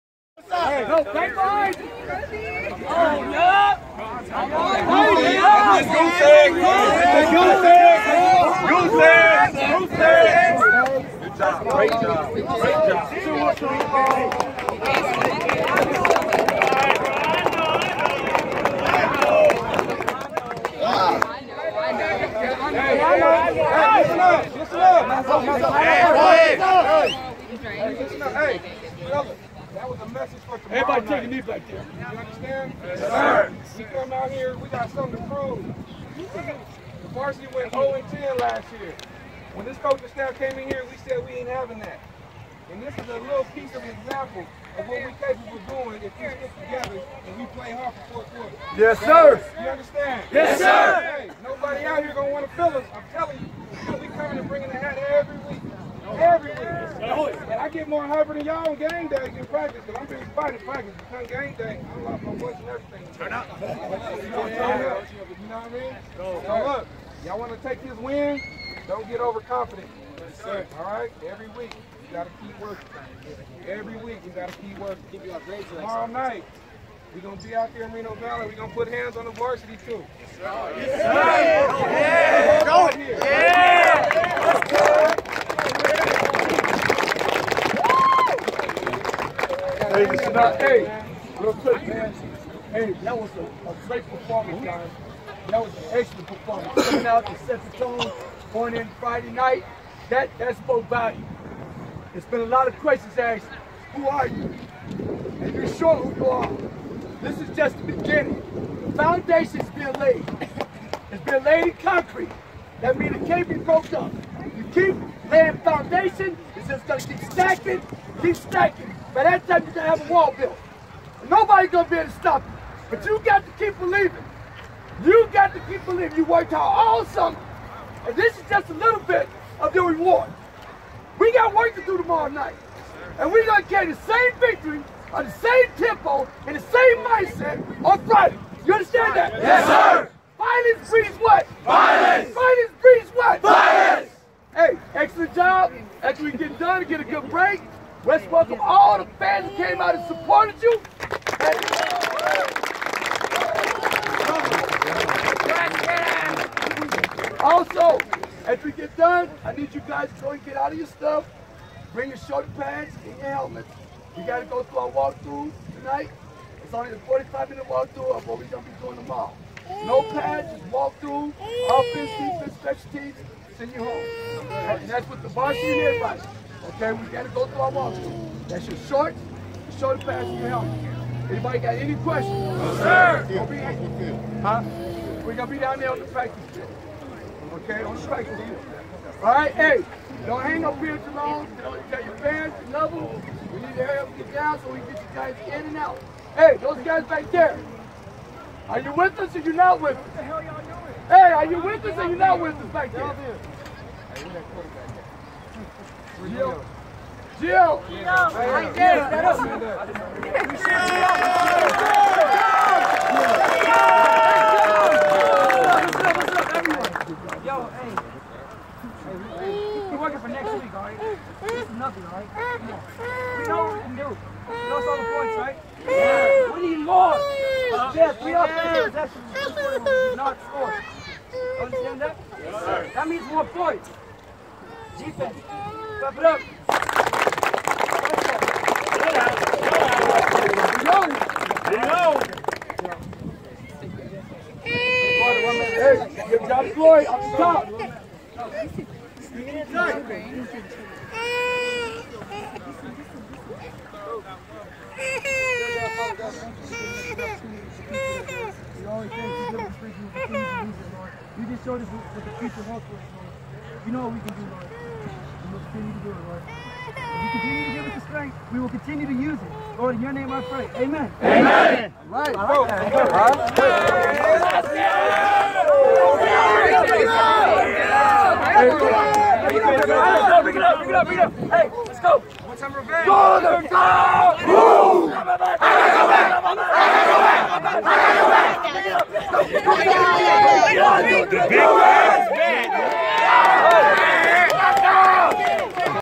Canyon Springs JV starts the season with a 26-0 win
New program, New philosophy, New Cougars! MoVal down this week. It doesn't matter who's next but we're ready